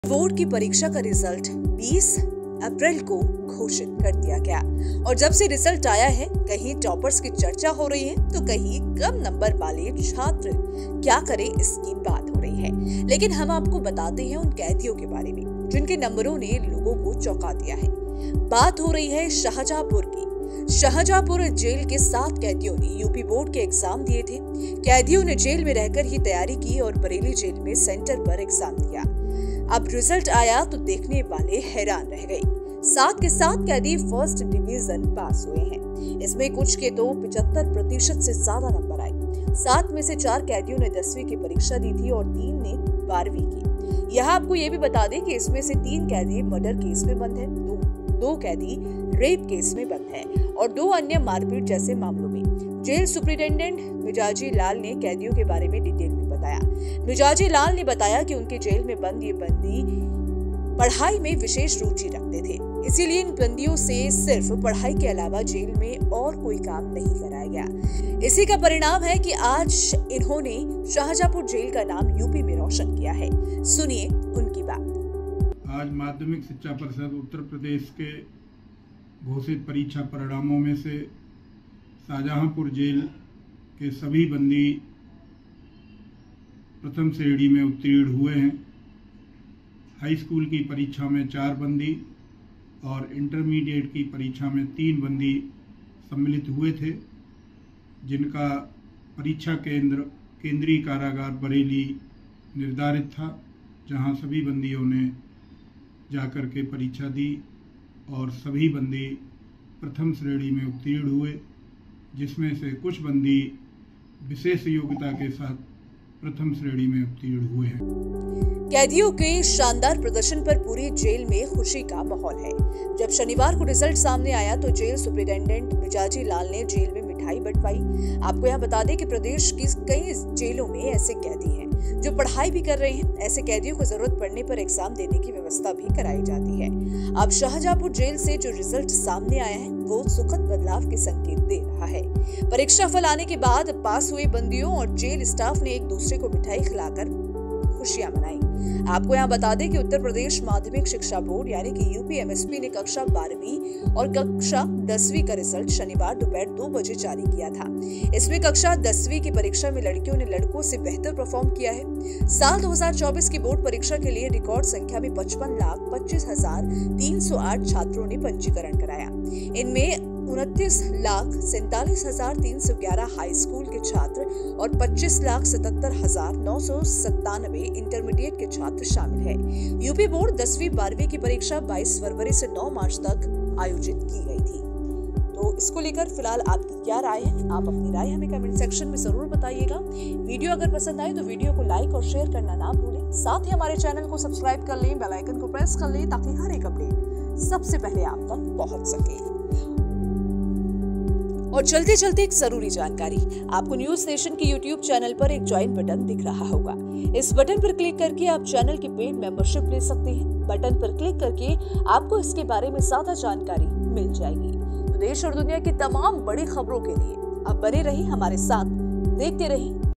UP Board Result 2024: Shahjahanpur की जेल में बंद कैदियों ने दिखाया कमाल, नंबर हैरान कर देंगे!
यूपी बोर्ड एग्जाम का रिजल्ट 20 अप्रैल को आया..जिसके बाद परीक्षा में टॉप करने वाले छात्रों की बात हो रही है.. लेकिन क्या आप जानते हैं कि जेल बंद कैदियों का रिजल्ट कैसा रहा. जानने के लिए देखिए वीडियो-

The result of UP board exam came on 20th April.. after which there is talk about the students who topped the exam.. but do you know how was the result of the jailed prisoners. Watch the video to know-